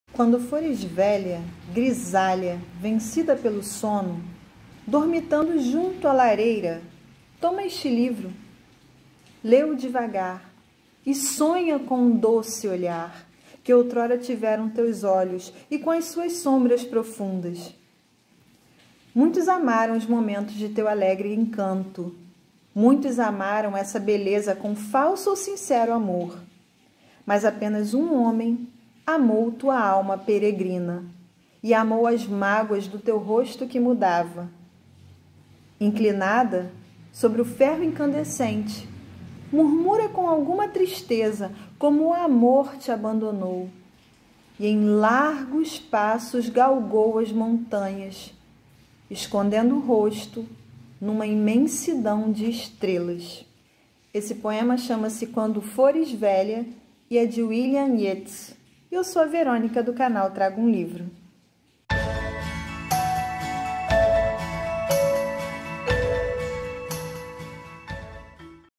Poesia Diária: Quando fores velha | William Yeats Leia
Na Poesia Diária de hoje, o Canal Trago um Livro apresenta o poema QUANDO FORES VELHA, de William Yeats. Um conselho com uma beleza triste para todos nós...